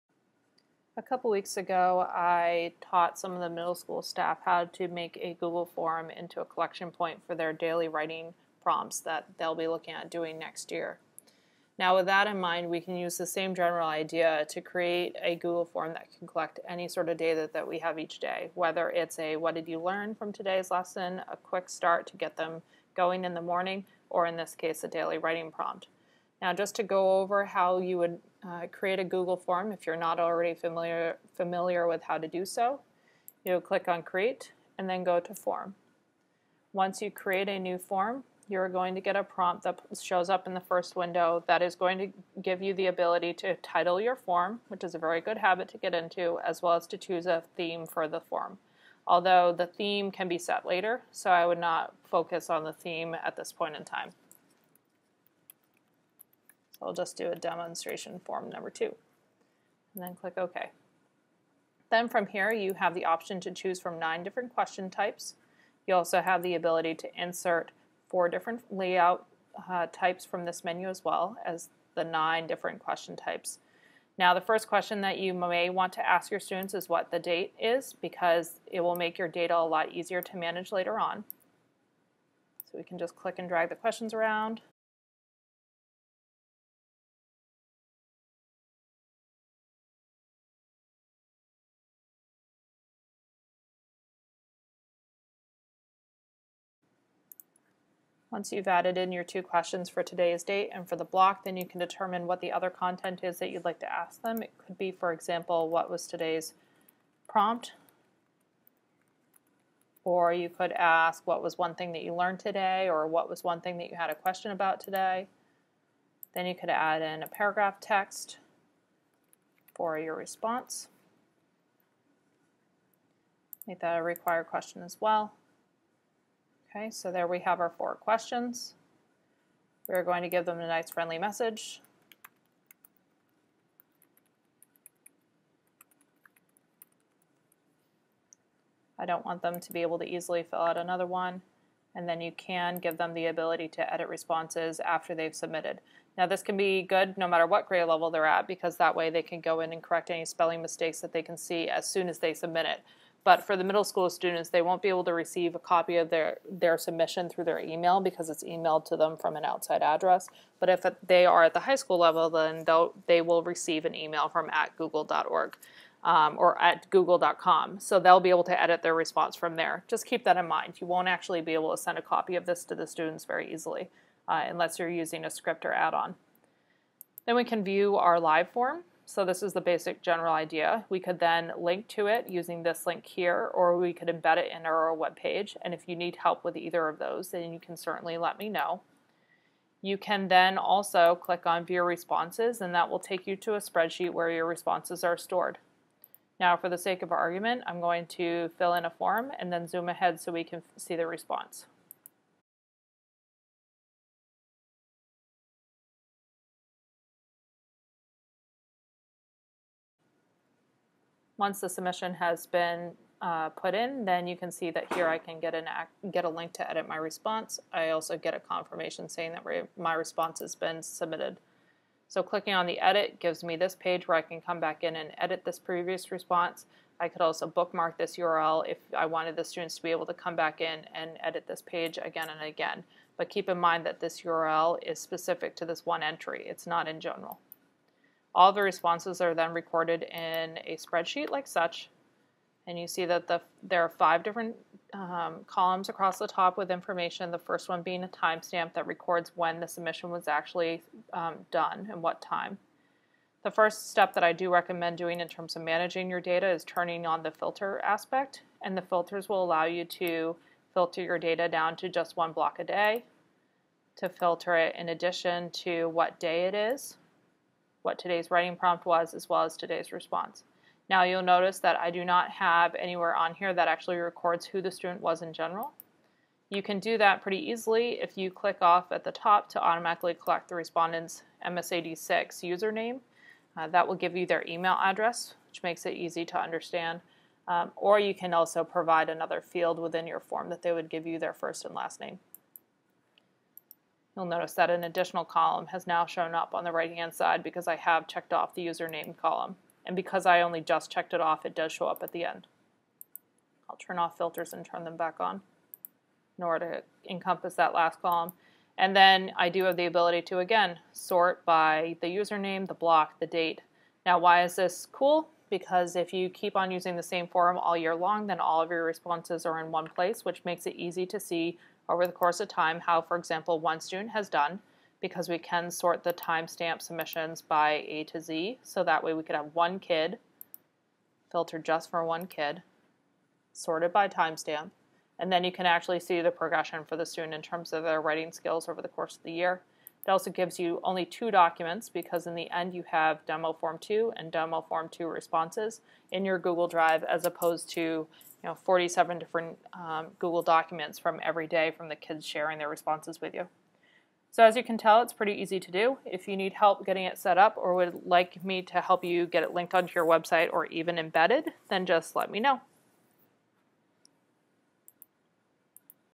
Using Google Forms for Daily Feedback and Responses
A quick overview of how to set up a Google Form to collect daily feedback in the classroom.